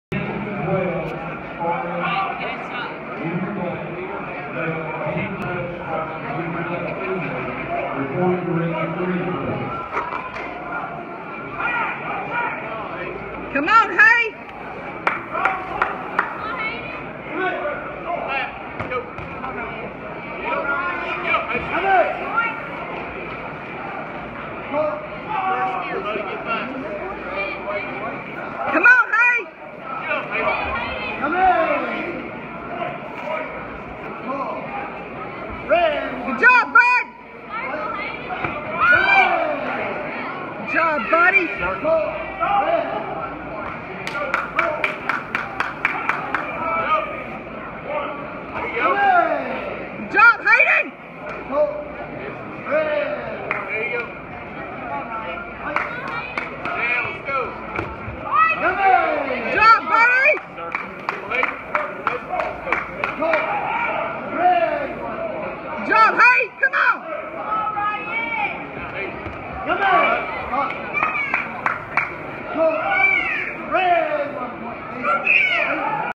Isshinryu Hall of Fame 2017/Fight #1
Hayden Cogdell wins 13-15 Age Division